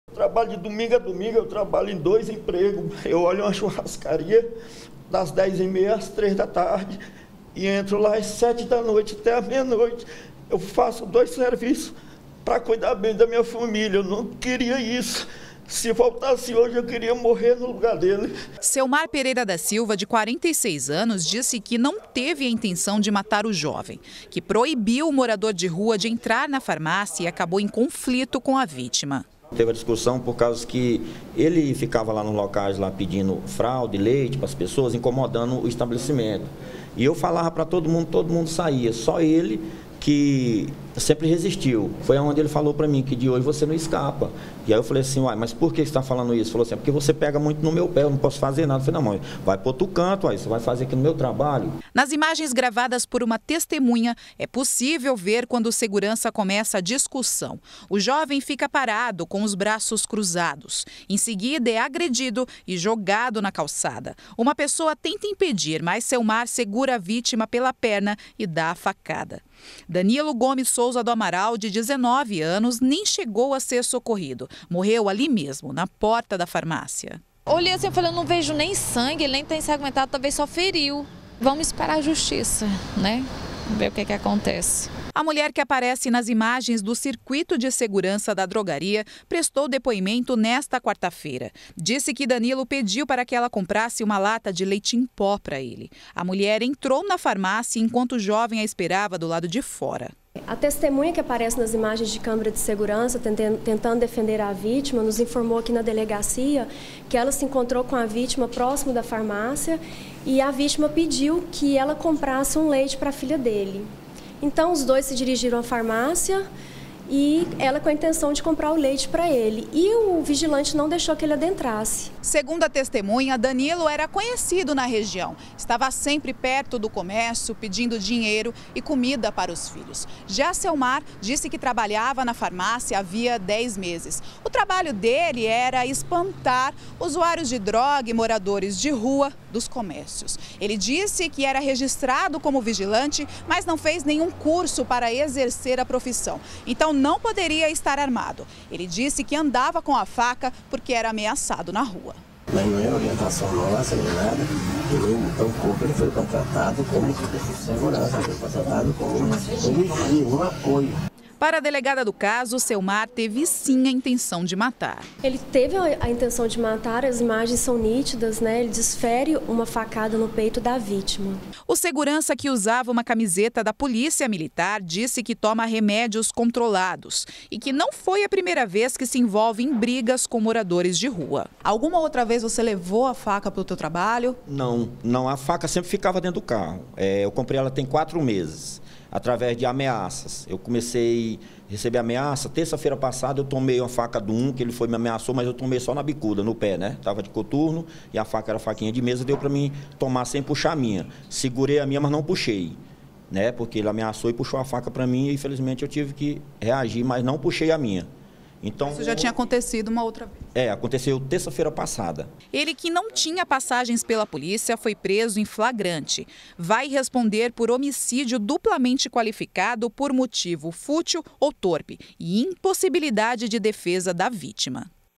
SEGURANÇA ESTÁ PRESO: CASO MORADOR DE RUA MORTO NA PORTA DE FARMÁCIA
E está preso o segurança que matou um morador de rua na porta de uma farmácia no Setor Leste Universitário. Ele vai responder por homicídio duplamente qualificado.